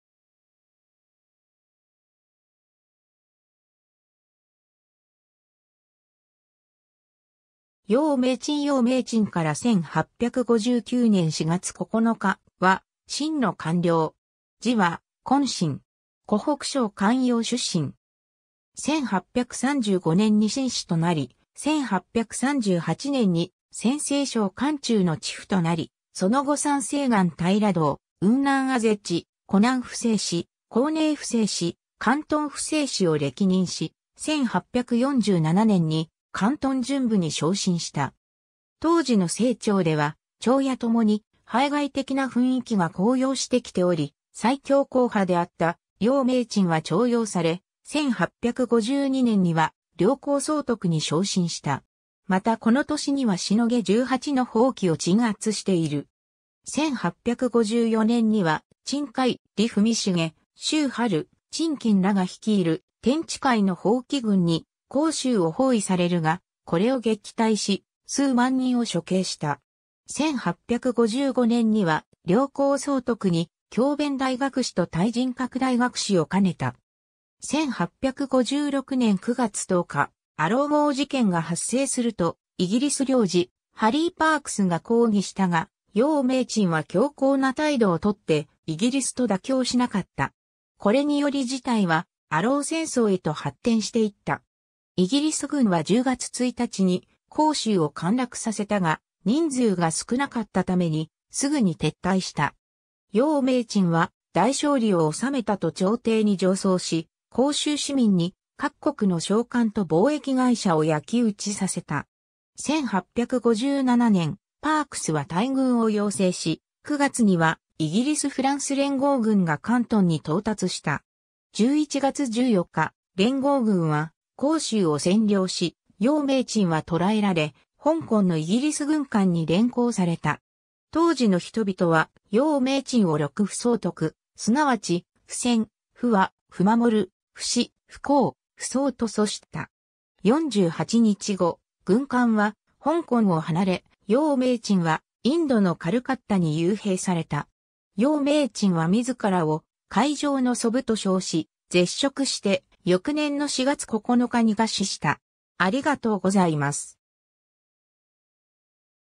葉名琛
葉名琛 葉 名琛（しょう めいちん、拼音: Yè Míngchēn、1807年12月21日（嘉慶12年11月23日） - 1859年4月9日（咸豊9年3月7日））は清の官僚。
字は崑臣。
 湖北省漢陽出身。
1835年に進士となり、1838年に陝西省漢中の知府となり、その後山西雁平道、雲南按察使、湖南布政使、江寧布政使、広東布政使を歴任し、1847年に広東巡撫に昇進した。
当時の清朝では朝野ともに排外的な雰囲気が高揚してきており、最強硬派であった葉名琛は重用され、1852年には両広総督に昇進した。
またこの年には凌十八の蜂起を鎮圧している。
1854年には陳開・李文茂・周春・陳金釭らが率いる天地会の蜂起軍に広州を包囲されるが、これを撃退し、数万人を処刑した。
1855年には両広総督に協弁大学士と体仁閣大学士を兼ねた。
 1856年9月10日、アロー号事件が発生するとイギリス領事ハリー・パークスが抗議したが、葉名琛は強硬な態度をとってイギリスと妥協しなかった。
これにより事態はアロー戦争へと発展していった。
イギリス軍は10月1日に広州を陥落させたが、人数が少なかったためにすぐに撤退した。
葉名琛は大勝利を収めたと朝廷に上奏し、広州市民に各国の商館と貿易会社を焼き討ちさせた。
 1857年、パークスは大軍を要請し、9月にはイギリス・フランス連合軍が広東に到達した。
11月14日、連合軍は広州を占領し、葉名琛は捕らえられ、香港のイギリス軍艦に連行された。
当時の人々は葉名琛を六不総督、すなわち「不戦・不和・不守・不死・不降・不走」とそしった。
 48日後、軍艦は香港を離れ、葉名琛はインドのカルカッタに幽閉された。
葉名琛は自らを「海上の蘇武」と称し、絶食して、翌年の4月9日に餓死した。